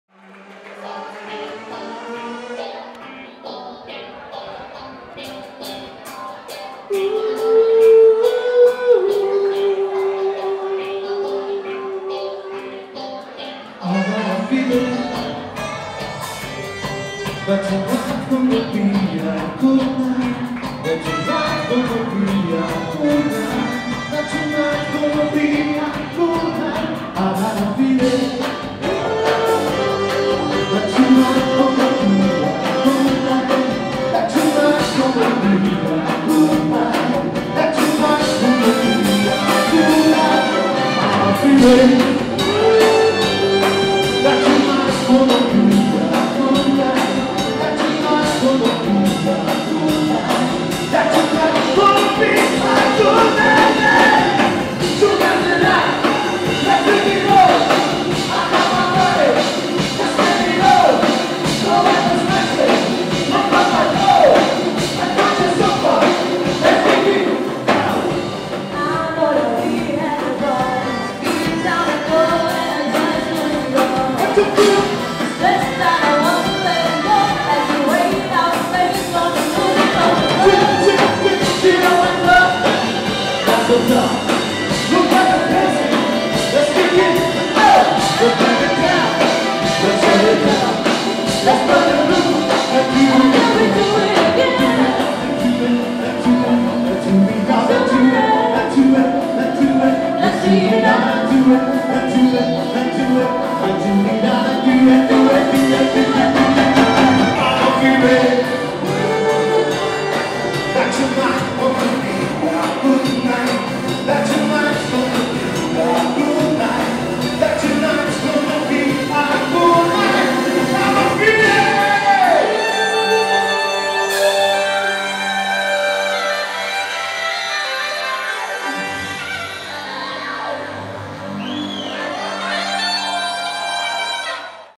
BANDA ÚNICA | Baile dos Amigos 2015
Um evento diferenciado onde nos apresentamos para familiares e pessoas que fizemos eventos e hoje são nossos amigos, foi uma noite especial e marcante, que energia , que clima!!! muito obrigado pela presença de todos!!!
Animação garantida para seu evento
Sensibilidade e emoção para sua Cerimonia !!!